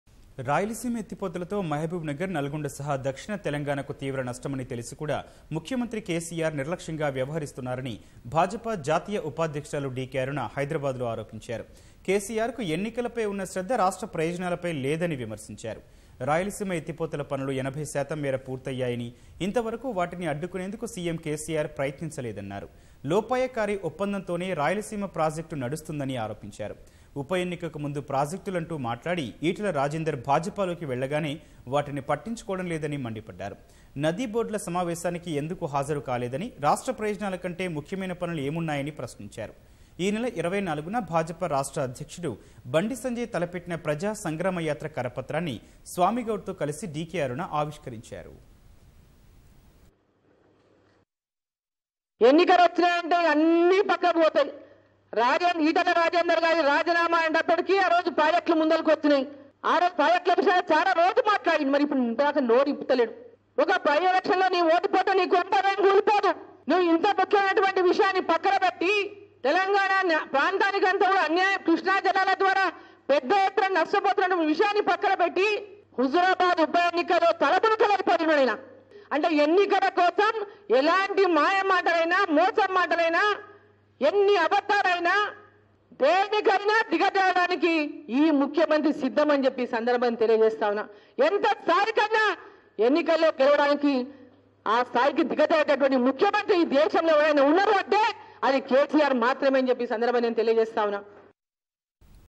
సీఎం KCRది నిర్లక్ష్య ధోరణి |CM KCR's Reckless Attitude in Rayalaseema Project | DK Aruna
రాయలసీమ ఎత్తిపోతలతో మహబూబ్ నగర్ , నల్గొండ సహా దక్షిణ తెలంగాణకు తీవ్ర నష్టం అని తెలిసి కూడా ముఖ్యమంత్రి KCR నిర్లక్ష్యంగా వ్యవహరిస్తున్నారని భాజపా జాతీయ ఉపాధ్యక్షురాలు DK అరుణ హైదరాబాద్ లో ఆరోపించారు. KCRకు ఎన్నికలపై ఉన్న శ్రద్ధ రాష్ట్ర ప్రయోజనాలపై లేదని విమర్శించారు. రాయలసీమ ఎత్తిపోతల పనులు 80 శాతం మేర పూర్తయ్యాయని... ఇంతవరకు వాటిని అడ్డుకునేందుకు CM-KCR ప్రయత్నించలేదన్నారు. లోపాయకారి ఒప్పందంతోనే రాయల సీమ ప్రాజెక్టు నడుస్తుందని ఆరోపించారు. ఉపఎన్నికకు ముందు ప్రాజెక్టులంటూ మాట్లాడి... ఈటల రాజేందర్  భాజపాలోకి వెళ్లగానే వాటిని పట్టించుకోవడంలేదని మండిపడ్డారు. నది బోర్డుల సమావేశానికి ఎందుకు హాజరు కాలేదని... రాష్ట్ర ప్రయోజనాల కంటే ముఖ్యమైన పనులు ఏమున్నాయని ప్రశ్నించారు. ఈ నెల 24 న భాజపా రాష్ట్ర అధ్యక్షుడు బండి సంజయ్ తలపెట్టిన ప్రజా సంగ్రామ యాత్ర కరపత్రాన్ని స్వామి గౌడ్ తో కలిసి DK అరుణ ఆవిష్కరించారు.